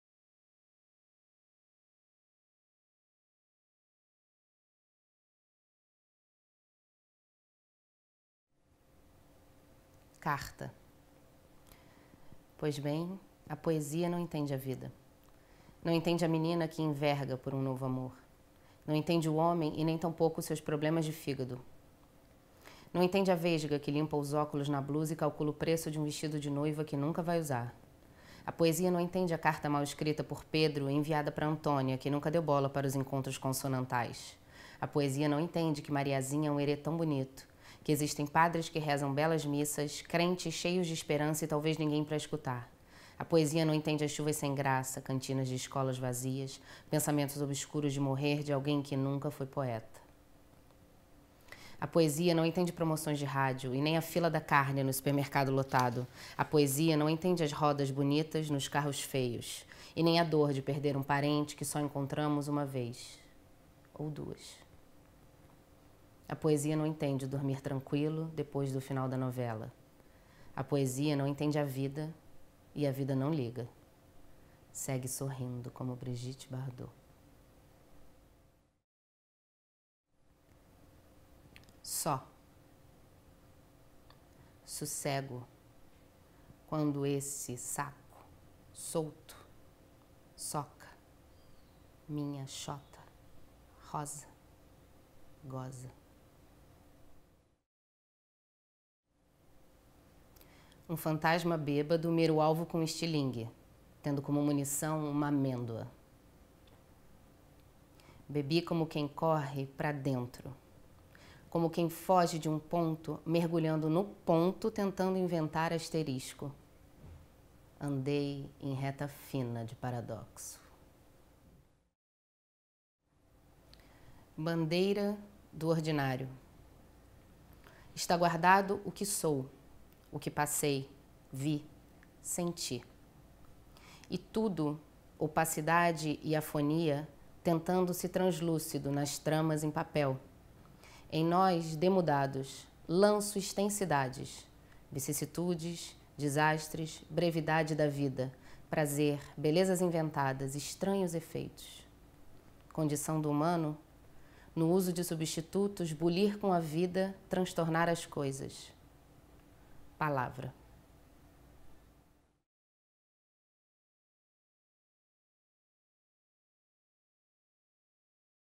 Porremas :: Karine Teles
Vinte autores e um mote: escreva muito bêbado, edite de ressaca. Organizada por Rafael Maieiro, Diego Barboza e Manuela Oiticica, “Porremas” é uma antologia poética que reúne 20 autores: Aldir Blanc, Alexandre Guarnieri, André Rodrigues, Bruno Borja, Daniel Brazil, Diego Barboza, Fabrício Gonçalves, Flora Tarumin, Jhone Carlos, Karine Teles, Katyuscia Carvalho, Léo Haua, Luiz Guilherme Santos, Manuela Oiticica, Mariana Blanc, Rafael Maieiro, Roberto Dutra Jr., Sandro Félix, Vinícius “Maru” Pitangui, Zeh Gustavo.

Neste vídeo, Karine Teles apresenta "Um fantasma bêbado mira o alvo com um estilingue; tendo como munição uma amêndoa" de Rafael Maieiro; "Só" de Flora Tarumim; "Carta" de Diego Barboza e "Bandeira do ordinário" de Sandro Félix.